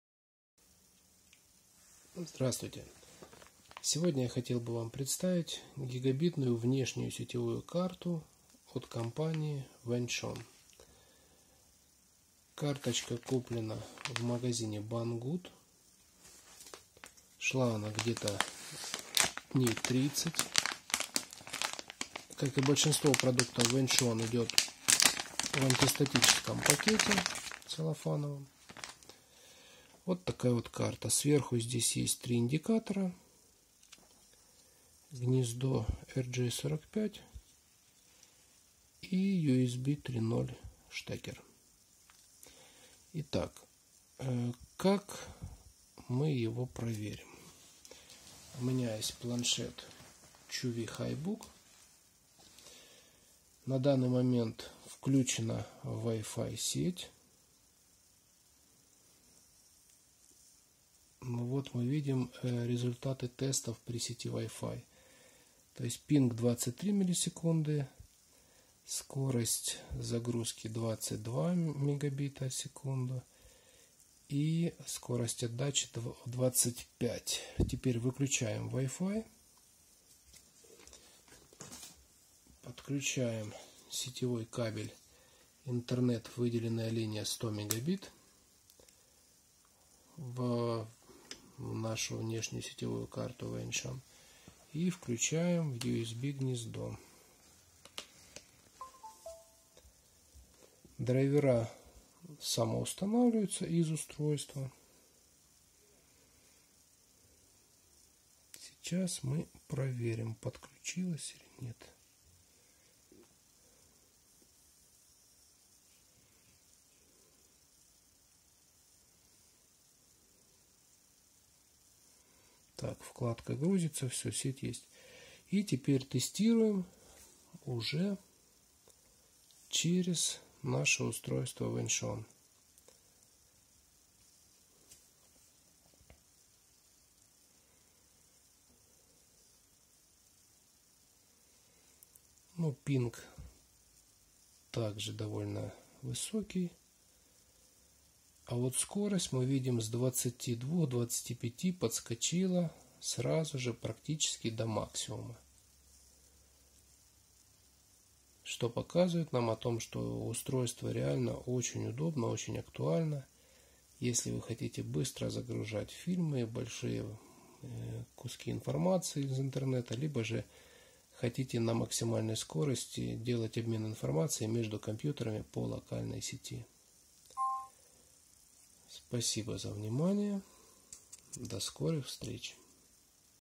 Внешний гигабитный USB3.0/LAN адаптер Vention
Адаптер куплен в магазине Banggood. Отличное качество от известного бренда.
Ссылка на товар
Flash deals